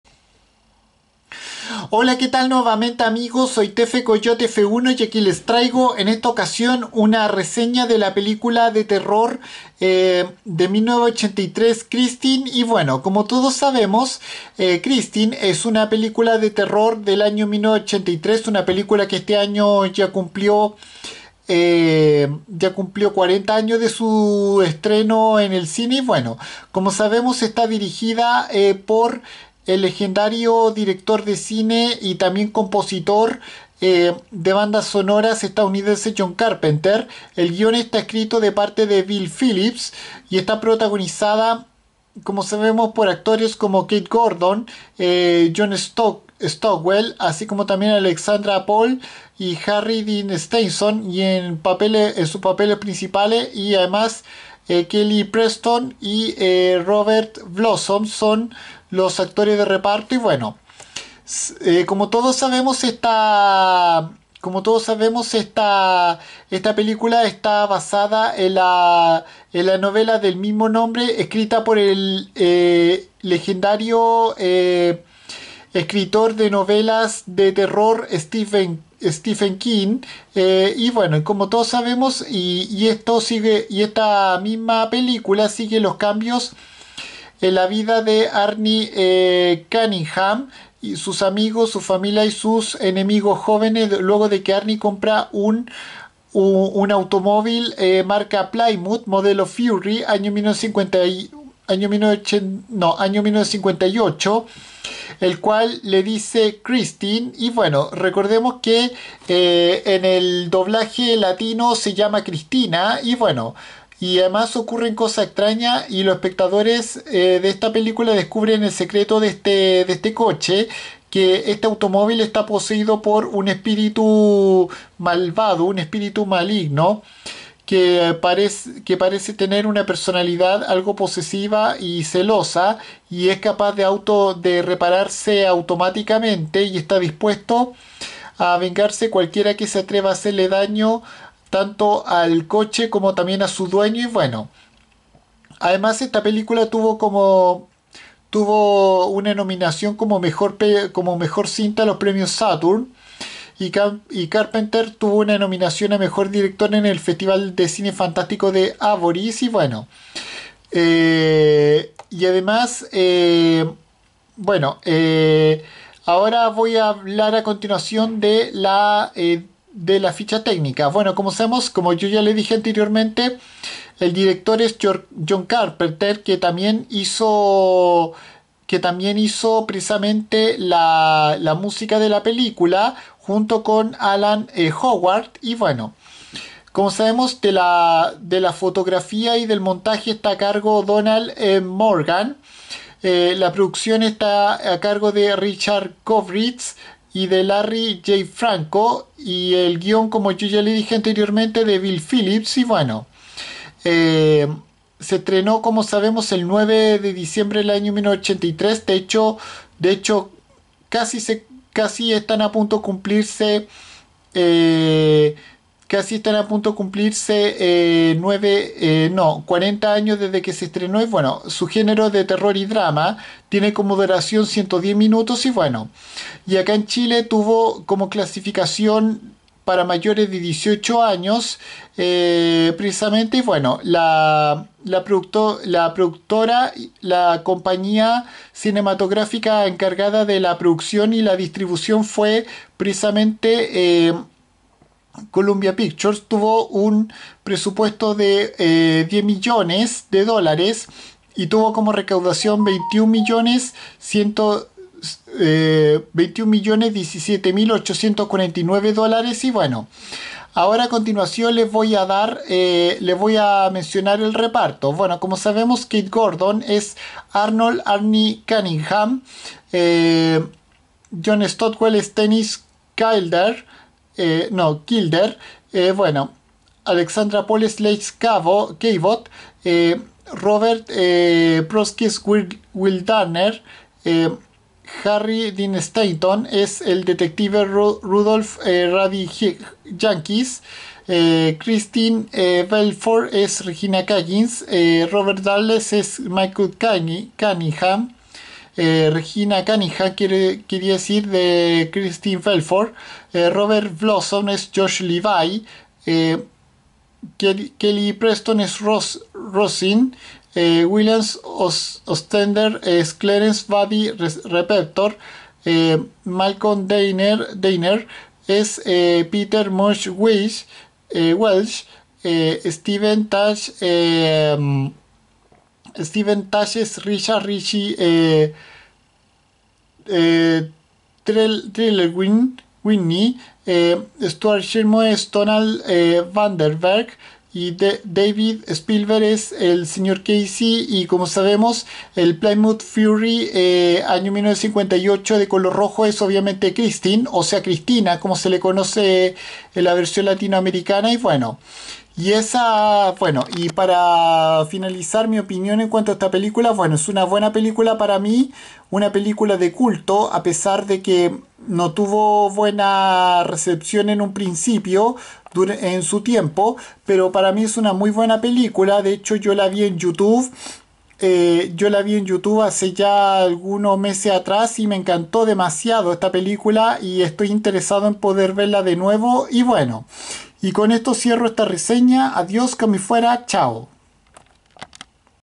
Reseña película Christine de John Carpenter de 1983
Esta película basada en la novela de terror del mismo título creada por Stephen King relata la vida de un joven estudiante de secundaria llamada Arnie el cual se compra un Plymouth Fury año 1958 el cual después se descubre que está poseído por un espíritu maligno